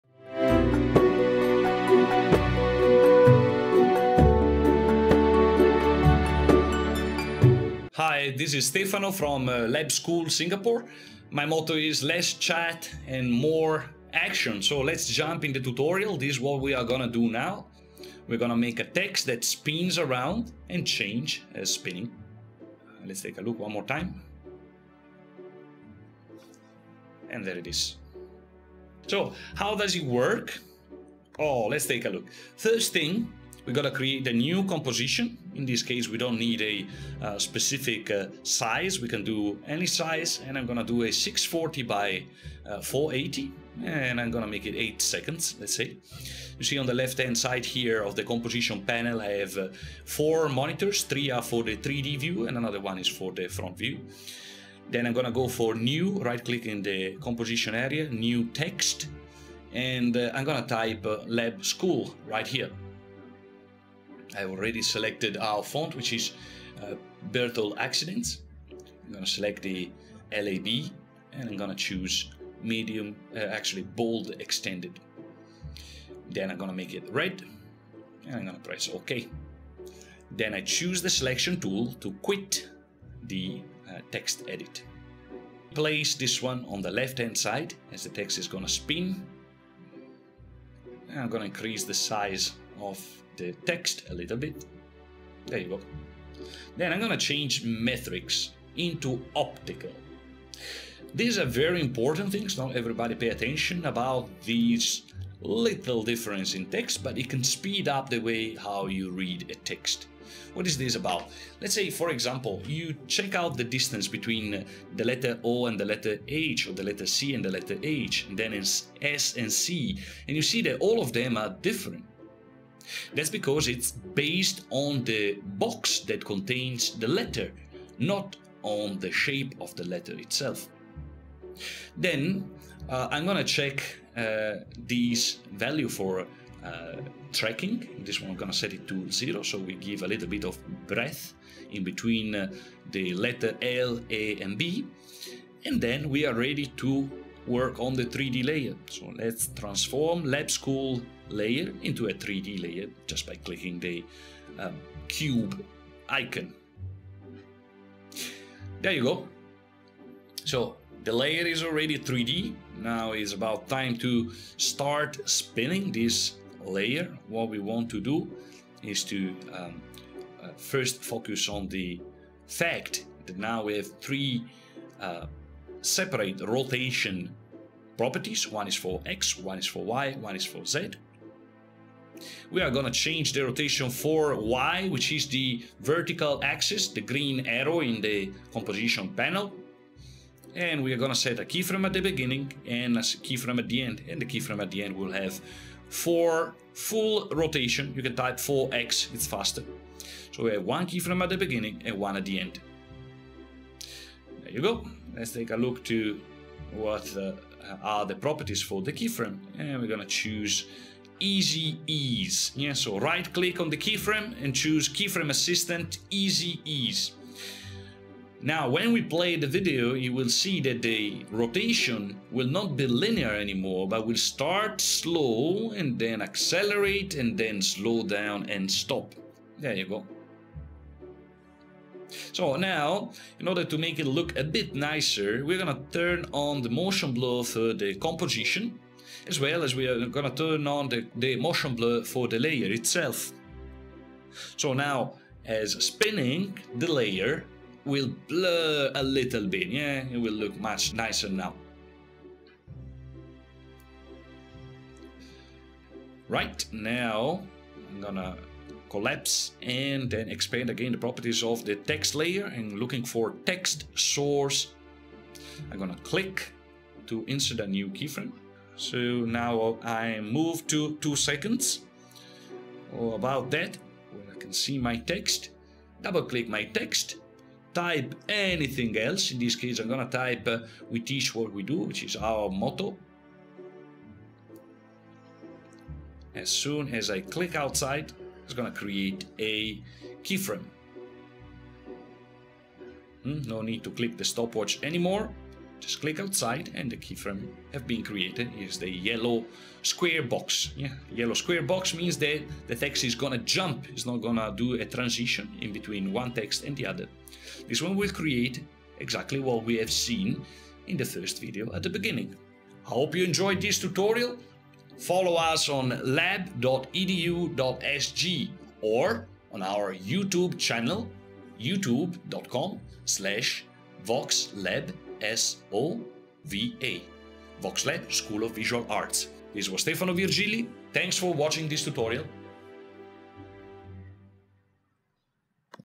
Adobe After Effects Video Tutorial - 3D rotating text by LAB School
Adobe Video Specialist Stefano Virgilli, director of Lab School Singapore, shows how to create a 3D rotating text, that changes at every rotation. Adding camera and lights can achieve a more impressive effect. High Definition tutorial.